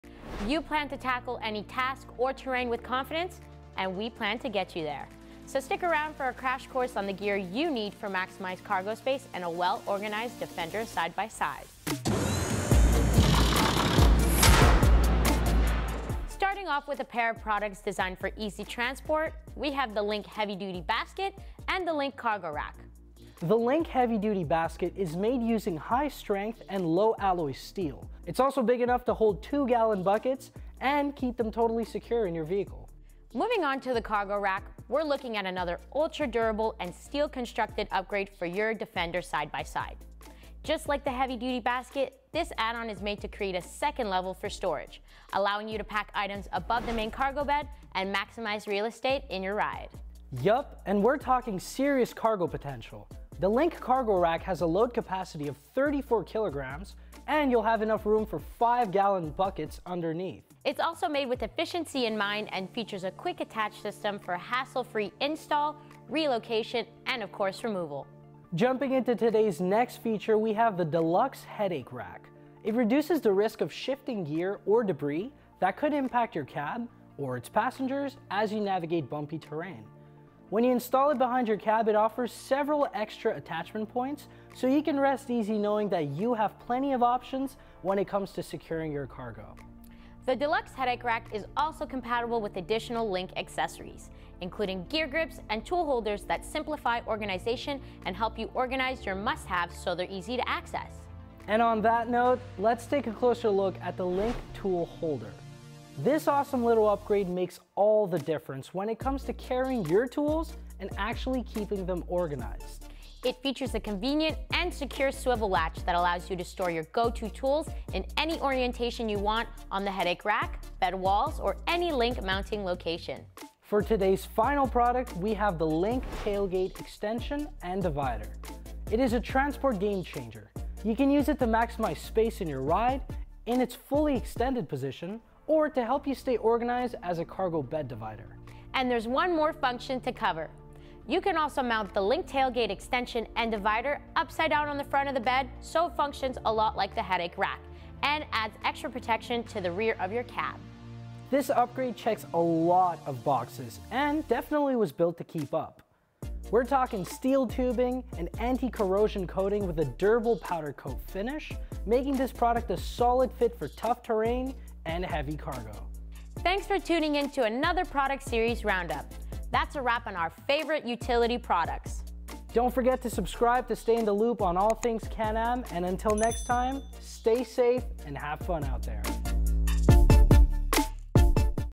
Defender Storage Accessories | Can-Am Product Series
For those who use a Can-Am Defender for big jobs & hard work, we've got a variety of functional products to increase cargo space and tackle any task.

To learn more about Off-Road Livin' and our lineup, plus more on how our products can optimize your utility tasks, visit our